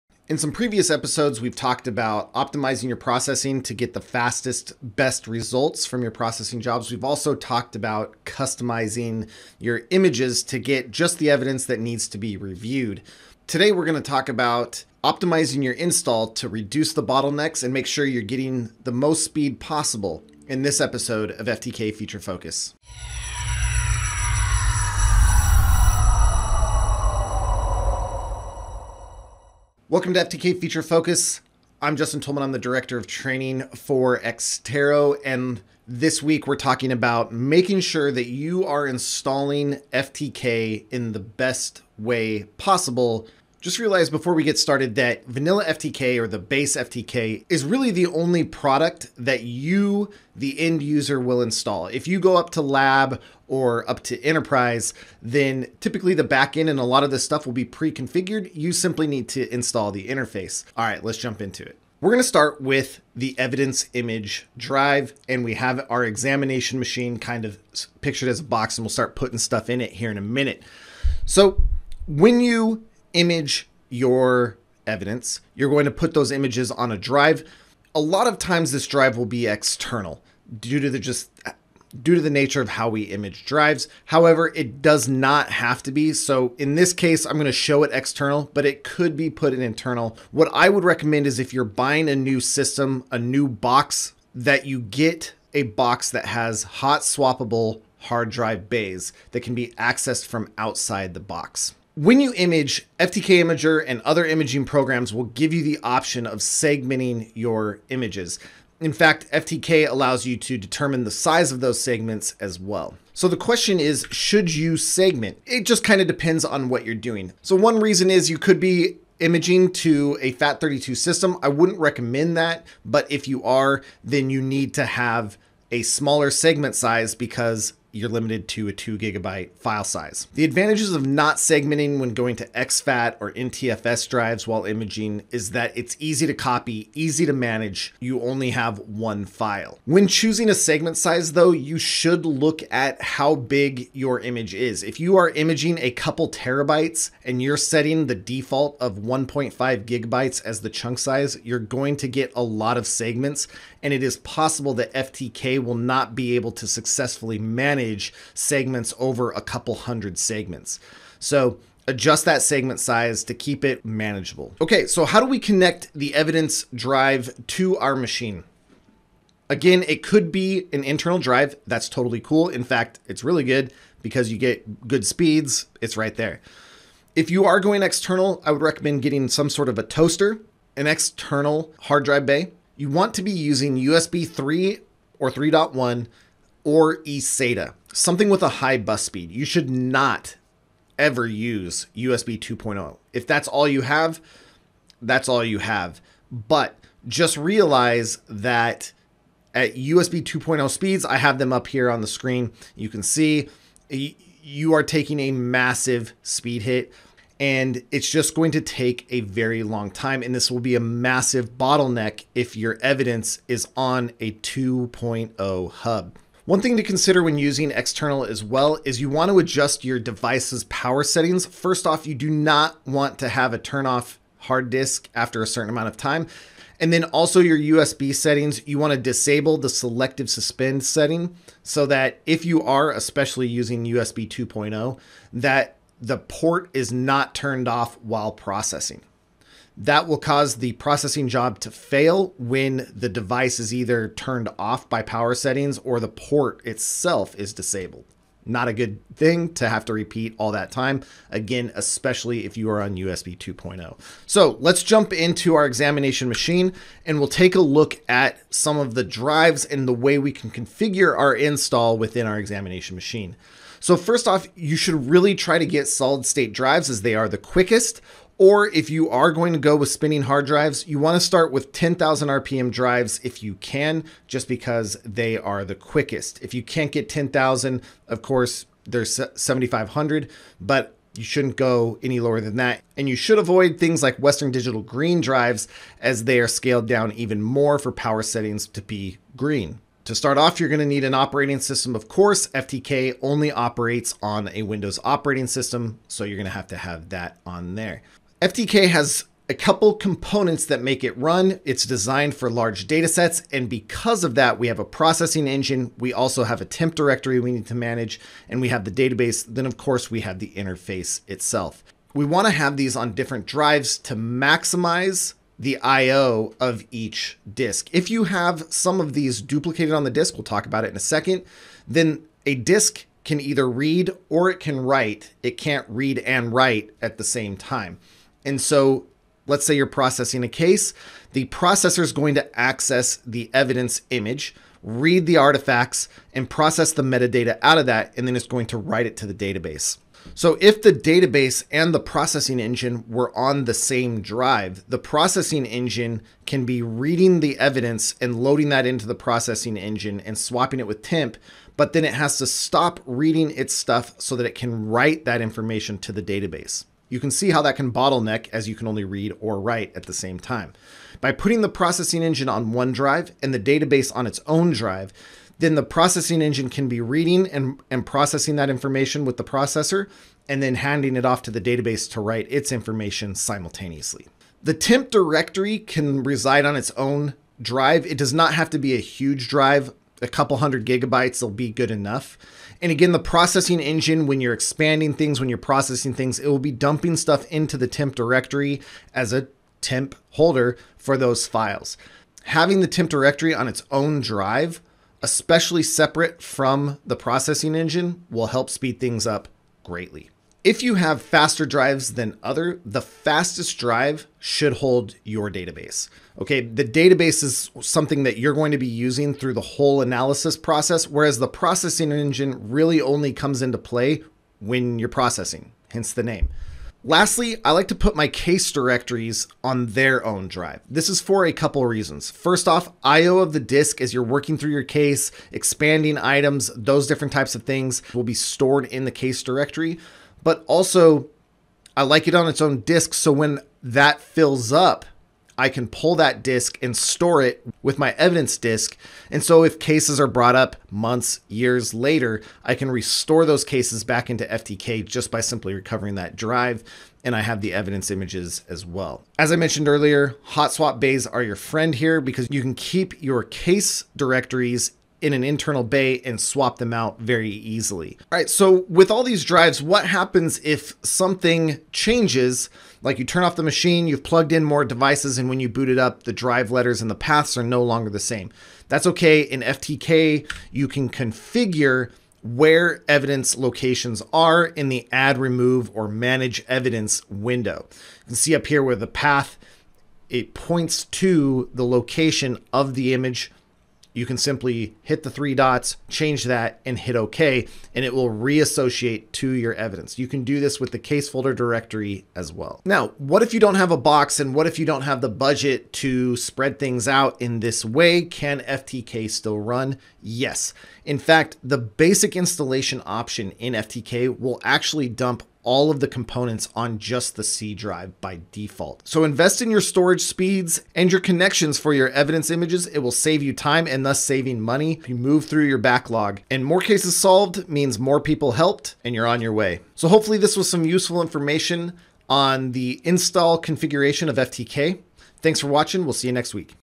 FTK Feature Focus - Episode 16 - Installation of FTK
FTK is designed for speed, but you can get the most out of Forensic Toolkit through proper installation of the different components used by FTK. This weeks video covers a quality installation map for base FTK.